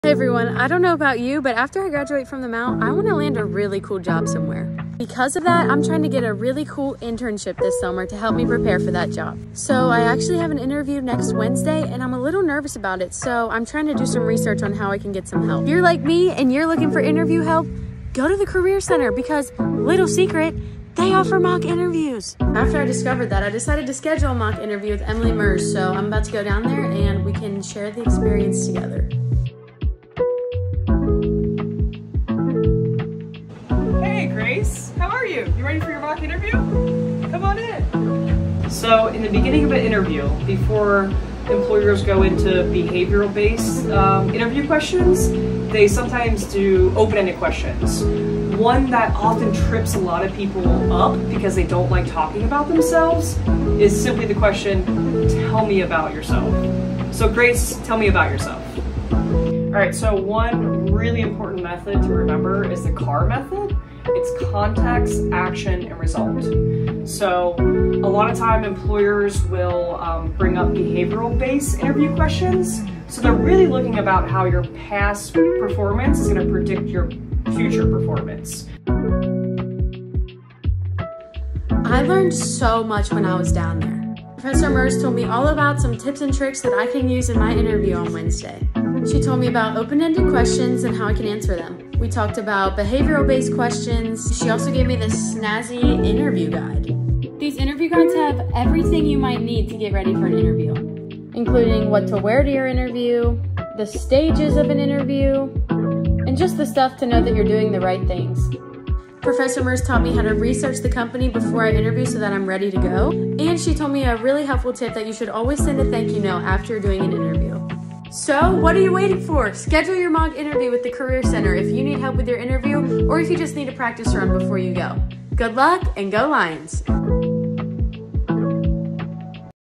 How can MSJ help me prepare for my career?
Follow along as sophomore student, Grace, prepares for an upcoming job interview. Grace ventures to our Career & Experiential Center to complete a mock interview where she goes through the experience of an interview while leaning tips and helpful skills that she use during her interview. See what she learns and how you can schedule a mock interview yourself!